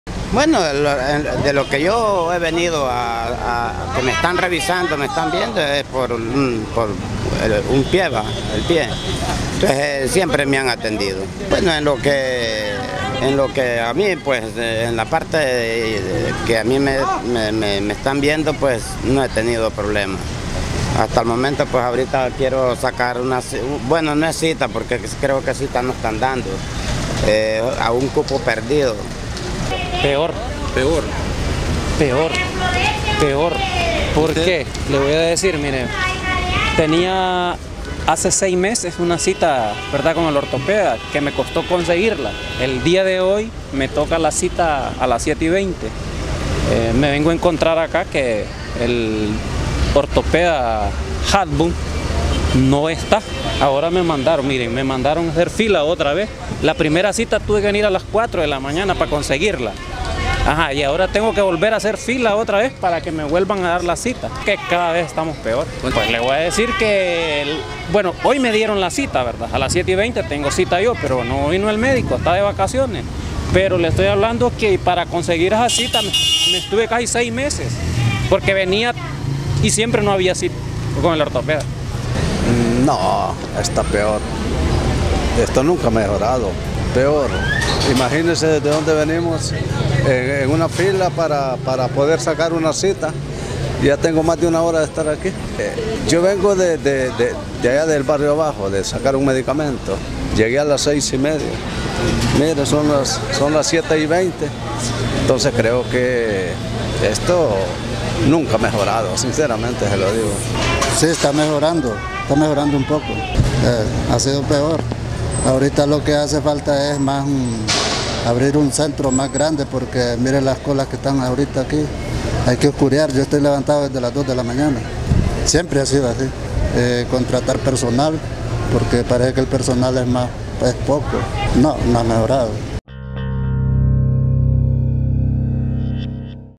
¿HA MEJORADO LA CALIDAD DE LA ATENCIÓN EN EL IHSS?
ElPulso En el Seguro Social del barrio La Granja le consultamos a varias personas que hacían fila para lograr conseguir una cita o un cupo perdido, si la atención ha mejorado o sigue igual, los pocos que se atrevieron a hablar contestaron que no.

Vea el video, comparta y opine, usted siente que hay mejoría con la junta interventora o sigue igual que antes.